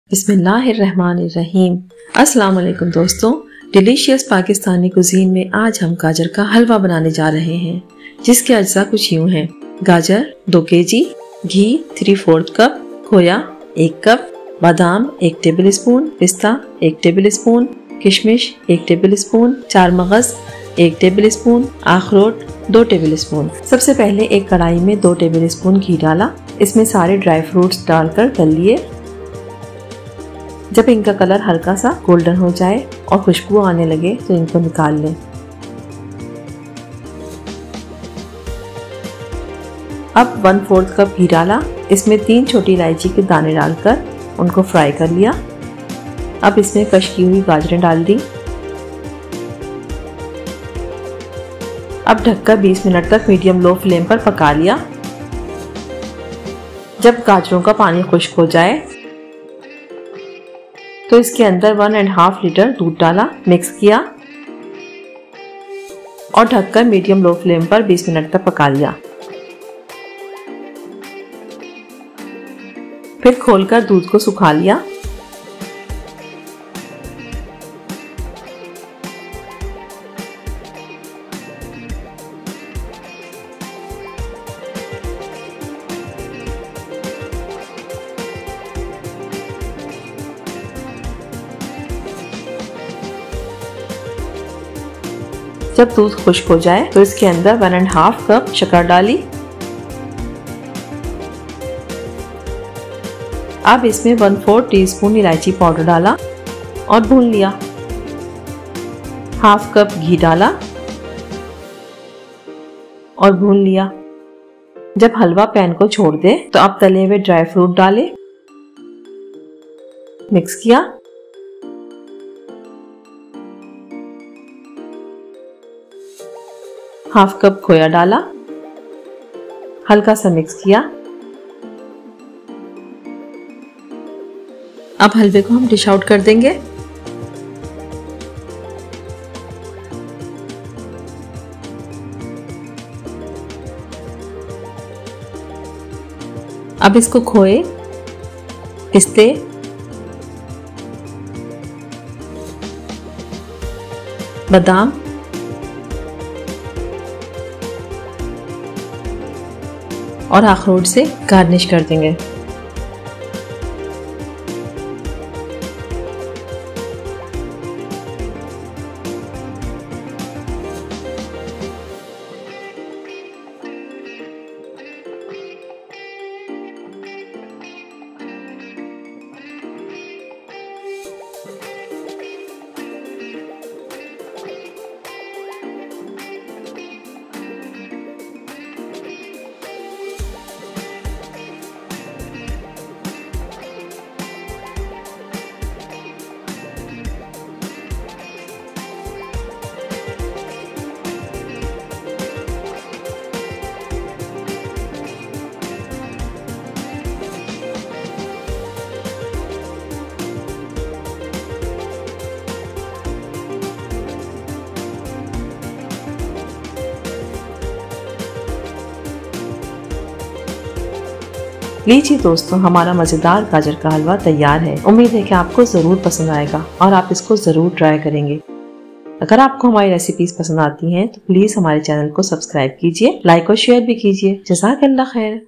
Gajar Ka Halwa Carrot Halwa delightful Recipe by Delicious Pakistani Cuisine
Gajar Ka Halwa Recipe by Delicious Pakistani Cuisine:

Ingredients:

Carrot (Gajar) 2 kg
Walnut (Akhrot) 2 tbsp
Raisins (kishmish) 1 tbsp
Dried Melon Seeds (Char magaz) 1 tbsp
Khoya 1 cup
Almond (Badam) 1 tbsp
Pistachio (Pista) 1 tbsp
Milk (Doodh) 1 ½ liters
Sugar (Shakr) 1 ½ cup

Procedure:

- In a wok add oil.
- Add all the dry fruits and fry them.
- Cook until the dry fruits change color and get fragrant. Set aside.
- In the same wok add ¼ cup oil.
- Add green cardamom seeds and fry for 30 seconds.
- Add julienne carrot and cook for 20 minutes at low flame until water dries.
- Add milk and mix it well.
- Cover and cook at the medium-low flame for 20 minutes until the carrot gets tender and water dries.
- Open the lid and cook at high flame until milk dries.
- Add sugar.
- Add green cardamom powder and mix.
- Add ½ cup ghee and mix it well.
- When the halwa separates from the pan then add half of the fried dry fruits and mix it well.
- Add khoya and mix.
- Dish out the halwa and garnish it with khoya, pistachio, almond, and walnut.
Gajar Ka Halwa (Carrot Halwa) is ready to serve.